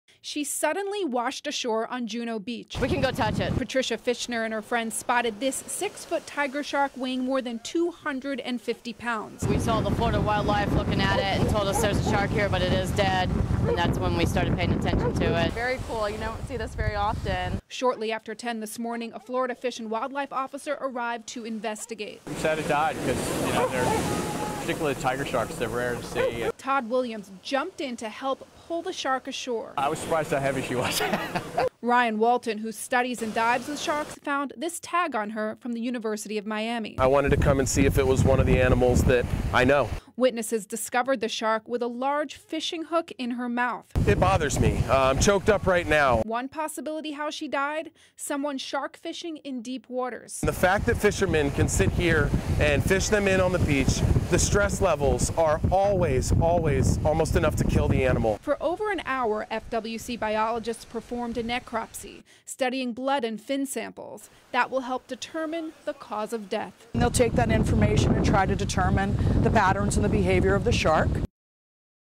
Dead shark washes ashore on Juno Beach
Florida Fish and Wildlife officer investigated and biologists performed a necropsy studying blood samples that will help determine the cause of death